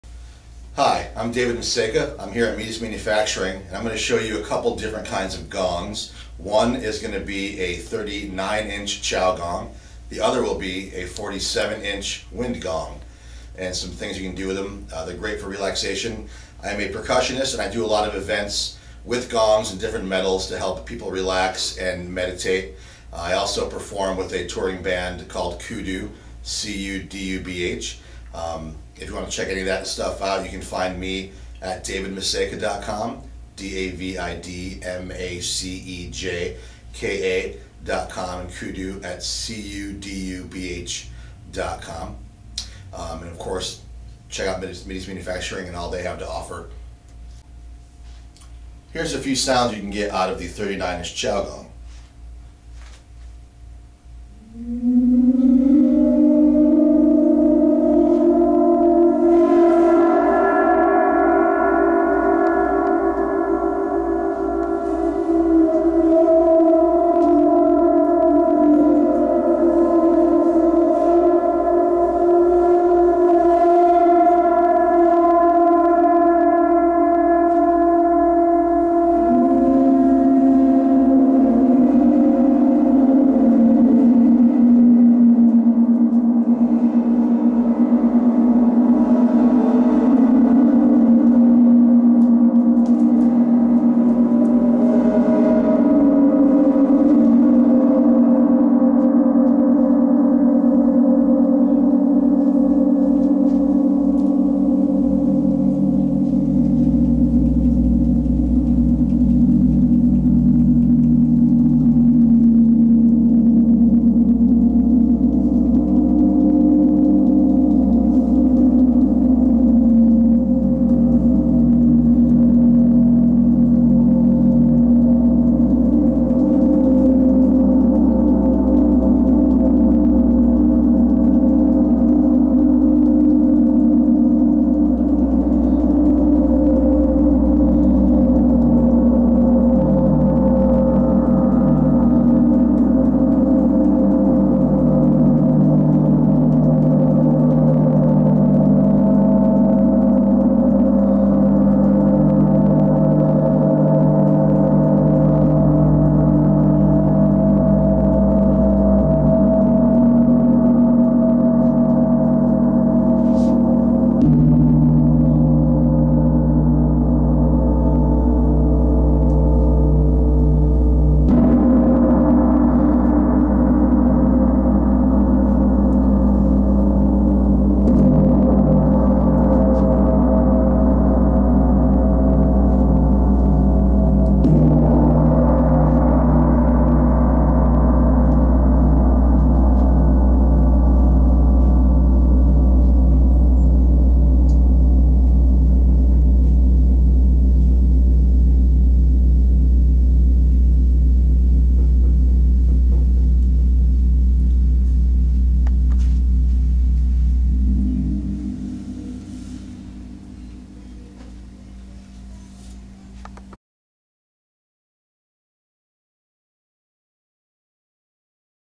WCG39-Chao Gong 39 Inch
The Chao Gong is a very elegant and distinctive gong that produces a full and sustained wash of musical overtones. Its beauty and its traditional appearance make it a centerpiece in any space, indoors or outdoors. This style of gong dates from the Han Dynasty where it was used for spiritual drumming, large meetings, and to announce the procession of important people. In more modern times, the chao gong has found a place in different genres of music, most notably symphony orchestras.
The Chao Gong, otherwise known as a tam-tam, is one of the most familiar looking Chinese gongs. The chao gong differs in shape from the wind gong in that it has a rim around the outside edge. It also has a very distinctive "target" appearance. It is made of a bronze alloy which is roughly 80% copper and 20% tin, and the crafting process is a mixture of hand hammering and working the bronze on a lathe while carefully heating and cooling the metal. On the chao gong, the center spot and outside rim retain the black copper oxide that forms when the gong is made. On the rest of the gong this coating is removed and the surface is polished. Chao gongs are available in a range of sizes, with each size having its own distinct full sound. Smaller sizes have a rich but higher sound while the larger sizes are deeper and ring with more power and sustain.